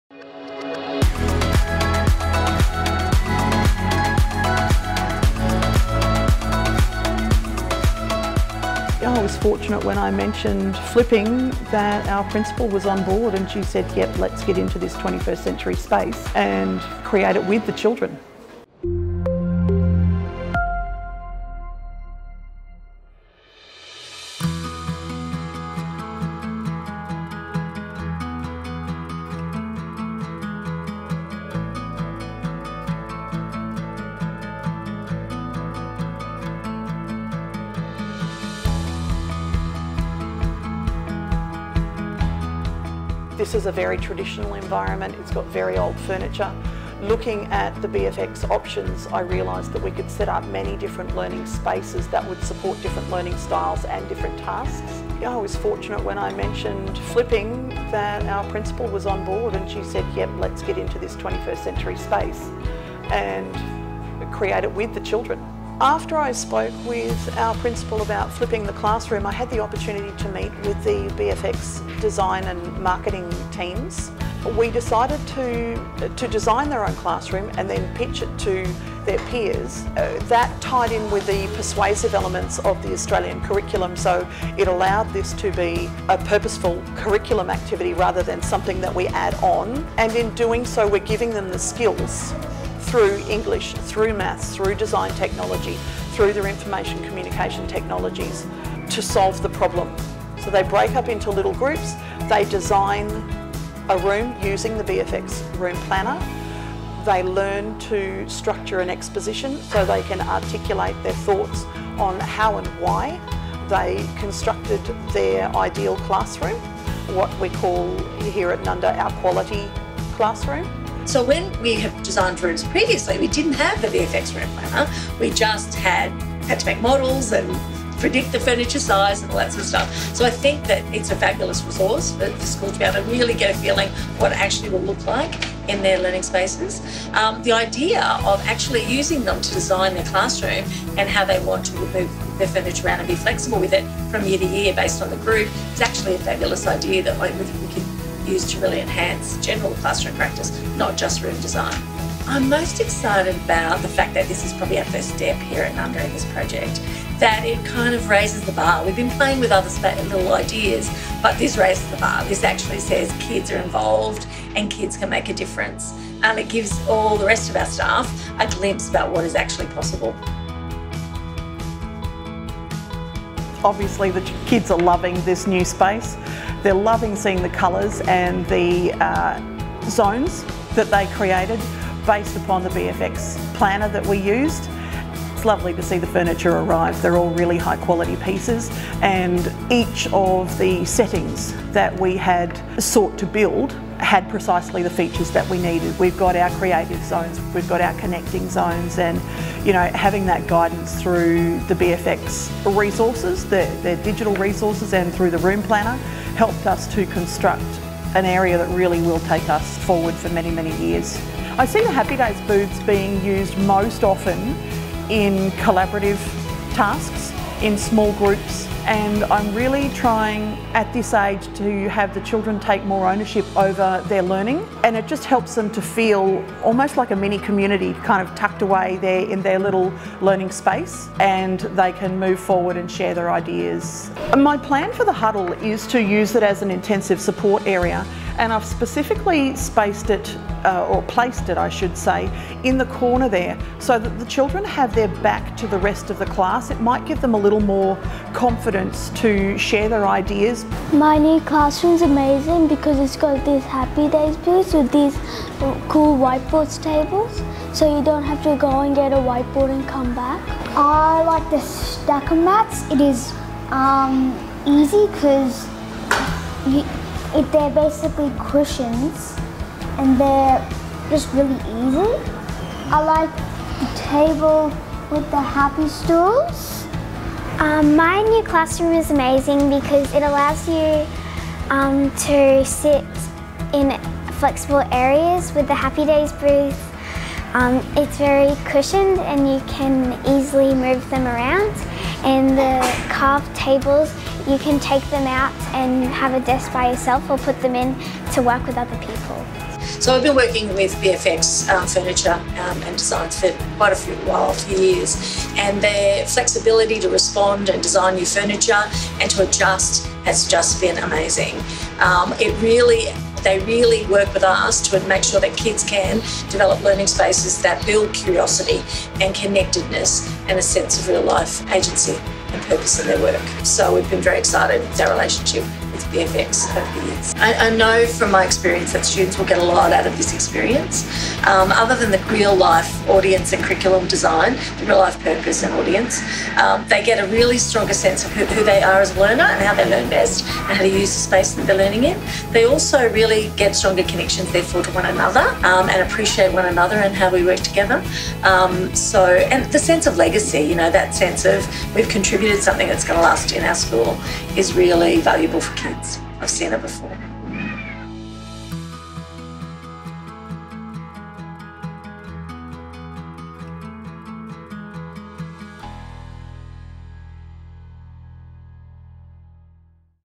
Student Classroom Design - Nundah State Primary - Case Study #9
See the new Nundah State Primary Grade 4 Classroom created with BFX 3D Roomplanner. Karen O'Connell, Grade 4 Teacher, shares her experience with the BFX 3D Room Planner in an exclusive interview.

With glowing consent and enthusiasm from Principal Deb Cox, and with help from the BFX team, she was able to start the journey.

The students were broken up into 7 groups and worked in teams to design their dream classroom using the BFX 3D Room Planner.
Learning how to use the technology was just one of the activities around classroom design they did.

Their teacher took them through different kinds of activities such as creating vision boards, drawing 2D floor plans, and writing proposals and speeches to present to the class.

The whole project engaged students from start to finish and the skills they demonstrated stretched across Maths, Design & Technology, English, Art and even Science.

To find out more about the activities the students did and even some suggested activities you might like to do with your students,